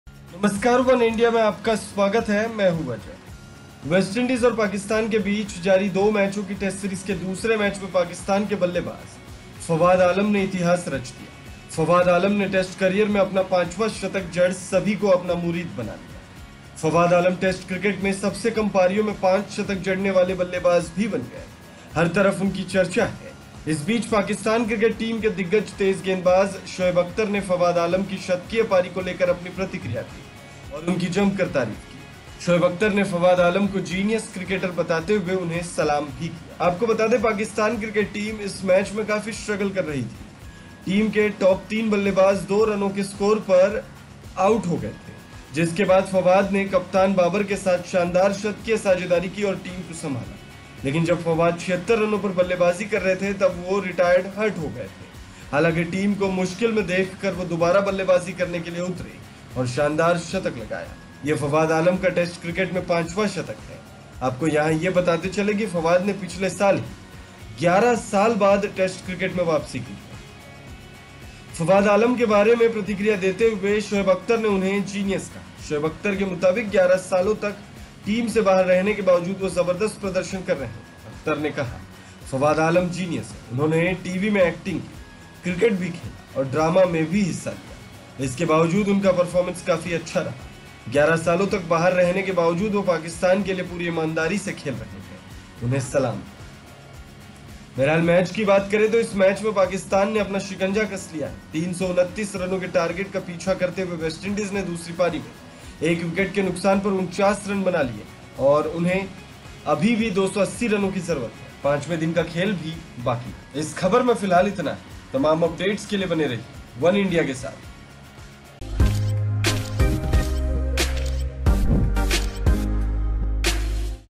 Shoaib Akhtar ने Fawad Alam की शतकीय पारी के बाद जमकर तारीफ की, कहा- 'सलाम है' | Oneindia Sports
Pakistan batsman Fawad Alam created history in the second match of the 2-match Test series between West Indies and Pakistan. Fawad Alam scored his fifth century in Test career and made everyone his admirer. Fawad Alam has also become the batsman to score 5 centuries in the shortest innings in Test Cricket. He is discussed everywhere, meanwhile Shoaib Akhtar, the veteran fast bowler of the Pakistan Cricket Team, has given his response to Fawad Alam's century innings and has also praised him fiercely. Shoaib Akhtar has also saluted Fawad Alam, describing him as a Genius Cricketer.

West Indies और Pakistan के बीच जारी 2 मैचों की Test series के दूसरे मैच में Pakistan के बल्लेबाज़ Fawad Alam ने इतिहास रच दिया। Fawad Alam ने Test करियर में अपना पांचवा शतक जड़ सभी को अपना मुरीद बना लिया। Fawad Alam Test Cricket में सबसे कम पारी में 5 शतक जड़ने वाले बल्लेबाज़ भी बन गए हैं। हर तरफ उनकी चर्चा है इस बीच Pakistan Cricket Team के दिग्गज तेज़ गेंदबाज़ Shoaib Akhtar ने Fawad Alam की शतकीय पारी को लेकर अपनी प्रतिक्रिया दी है और उनकी जमकर तारीफ़ भी की है। Shoaib Akhtar ने Fawad Alam को Genius Cricketer बताते हुए उन्हें सलाम भी किया है।